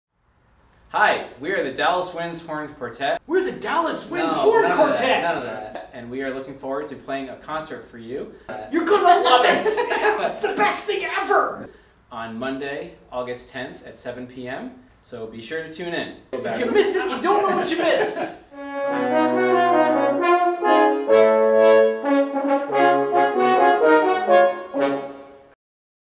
Dallas Winds Summer Serenade #5 - Horn Quartet
The Dallas Winds' Summer Serenade series continues Monday, August 10th (7:00pm CDT) with a live online concert featuring heroic Horn Quartet! $10 per household.